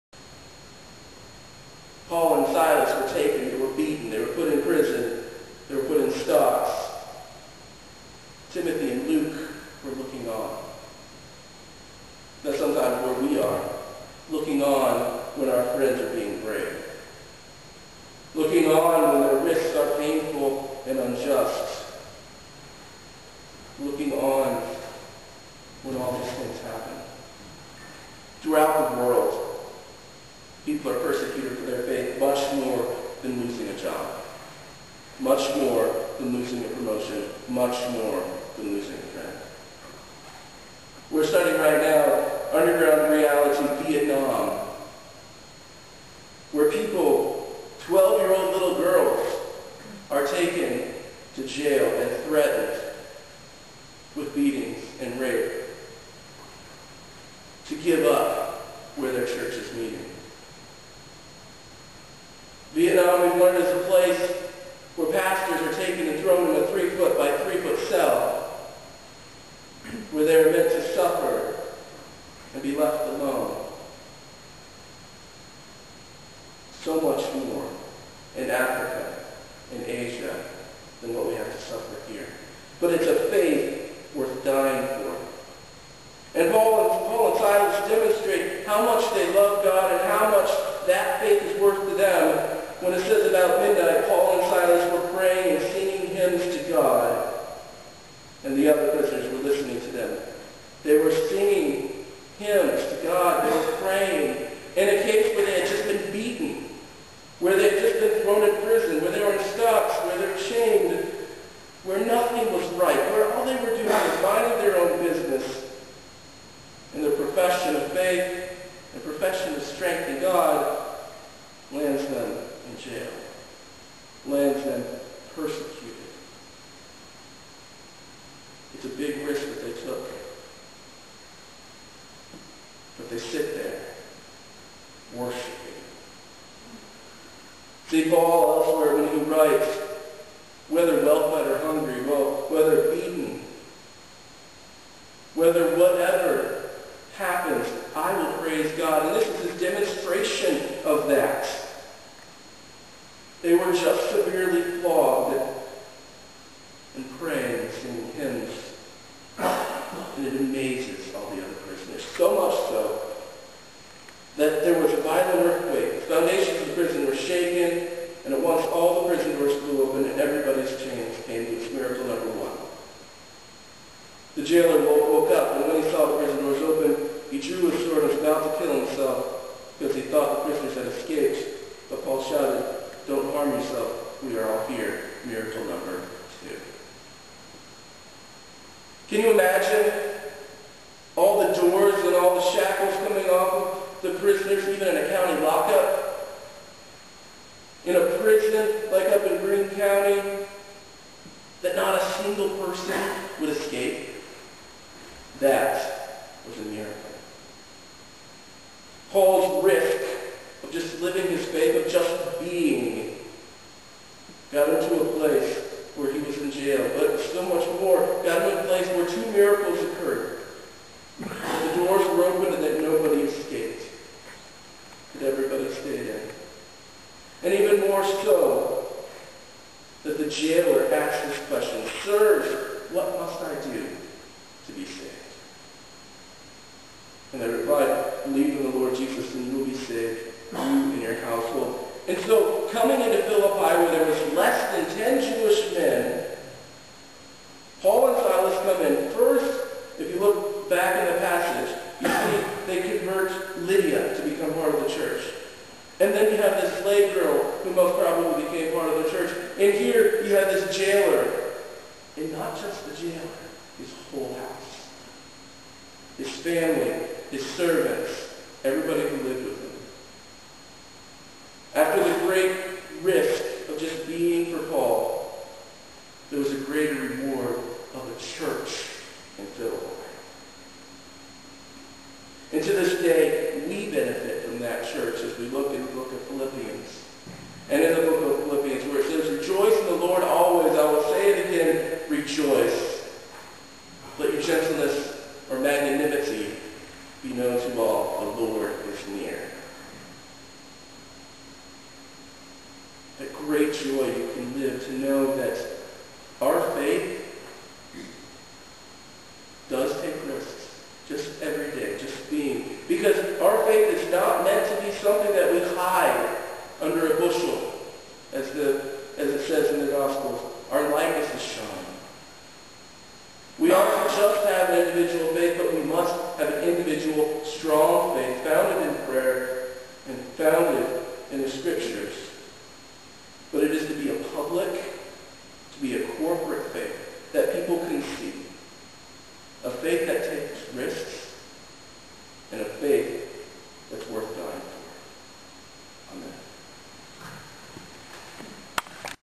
May 16th 2010 Sermon Part, Trinity Church, Pass Christian, Mississippi
Fr. Blauvelt concludes his sermon on May 16th 2010.